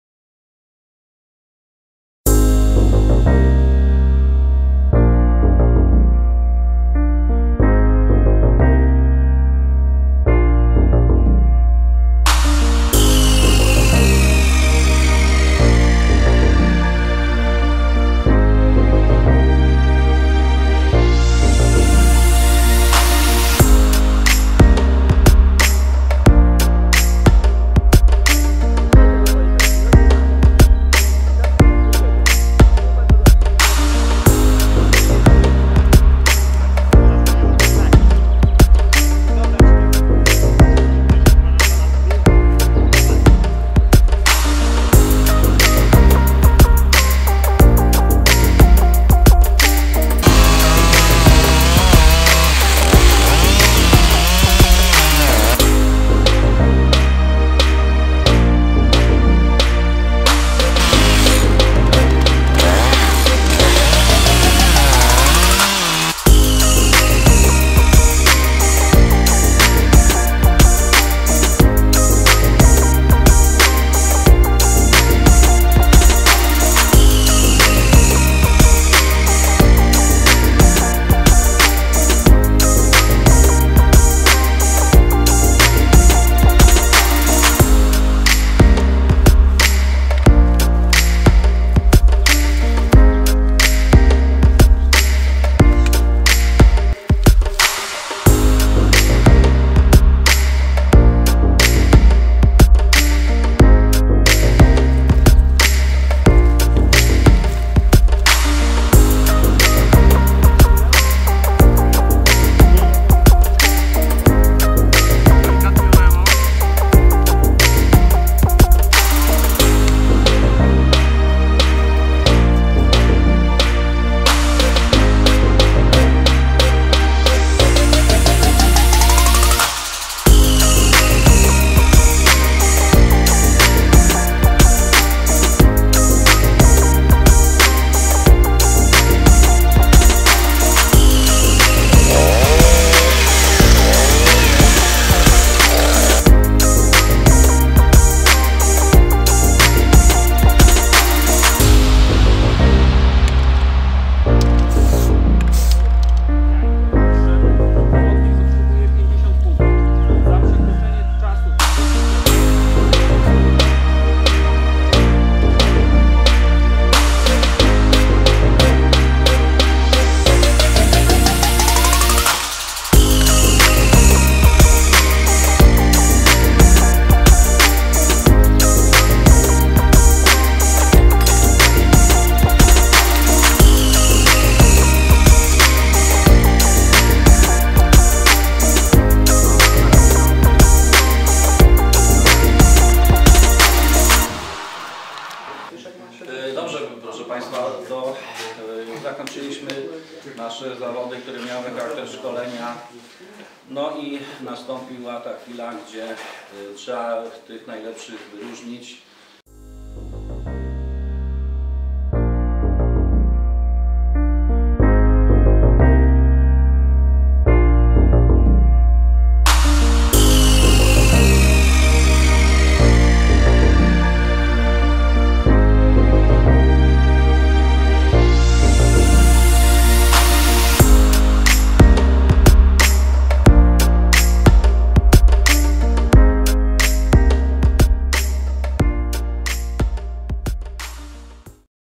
Zawody drwali 2018
Po raz kolejny na naszym terenie w formie zawodów odbyło się praktyczne szkolenie dla Zakładów Usług Leśnych z zakresu przestrzegania przepisów BHP podczas pozyskania drewna w lesie. W szkoleniu wzięło udział 15 pilarzy, którzy pokazywali swe umiejętności w 5 dziedzinach:

1. Ścinka drzew
2. Okrzesywanie drzew
3. Przygotowanie pilarki do pracy
4. Złożona przerzynka kłód
5. Dokładność przerzynki

Nad prawidłowym przebiegiem zmagań, bezpiecznym przebiegiem konkurencji oraz dokonywaniem pomiarów czuwał zespół oceniający złożony z pracowników Służby Leśnej Nadleśnictwa Leżajsk, którzy mierzyli i oceniali wykonywane czynności.